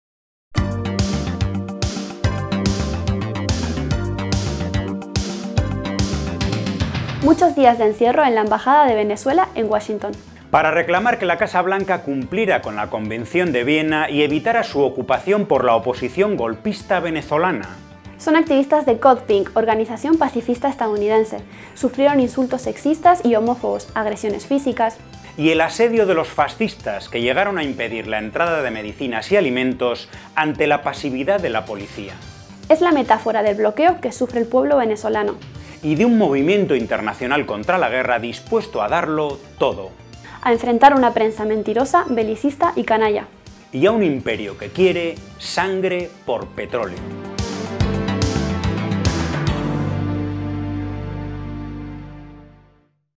Asedio a una embajada, asedio a un país
Son activistas de Code Pink, organización pacifista estadounidense. Que sufren el asedio de los fascistas opositores venezolanos en la embajada de Venezuela en Washington. Es la metáfora del bloqueo que sufre el pueblo venezolano. Y de un movimiento internacional contra la guerra dispuesto a darlo todo. Espacio "Contratuit", en Cubainformación TV.